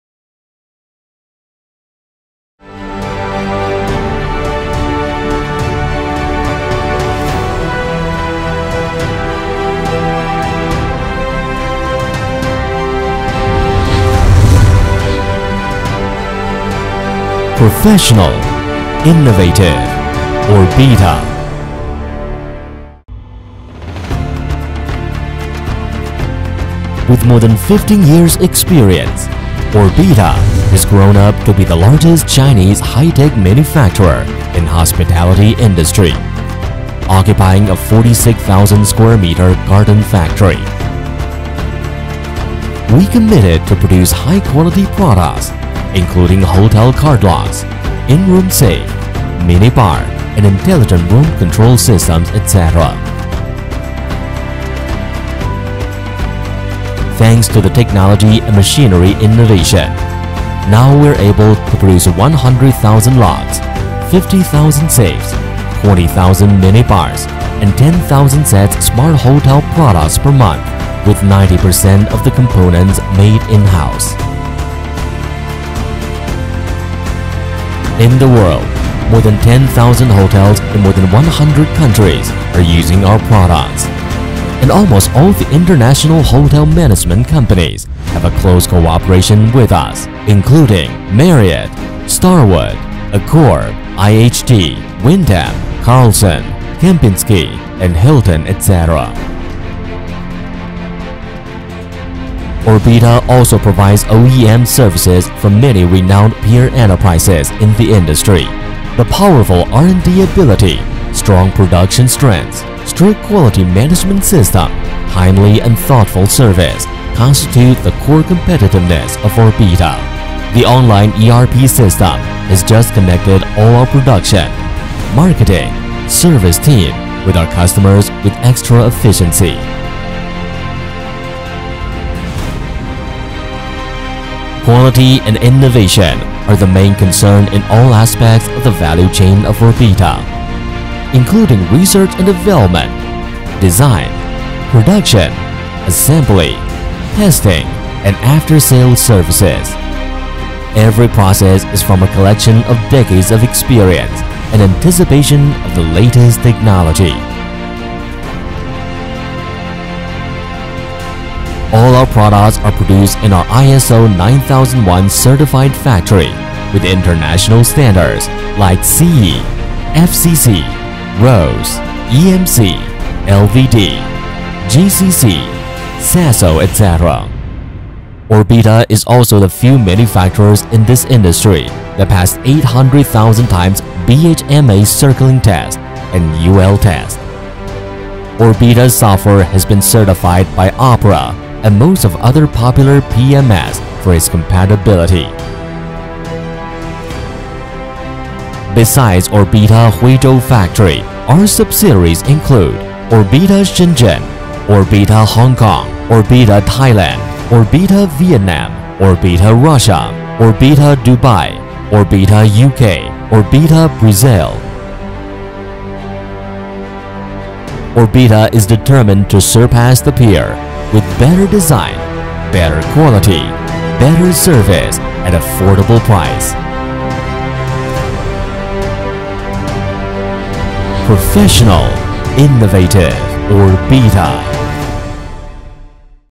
orbita company video more
Orbita is a reputable hospitality products manufacturer with ISO certified 46,000 square meters garden factory, established to offer the best quality products with professional and effective service to our customers.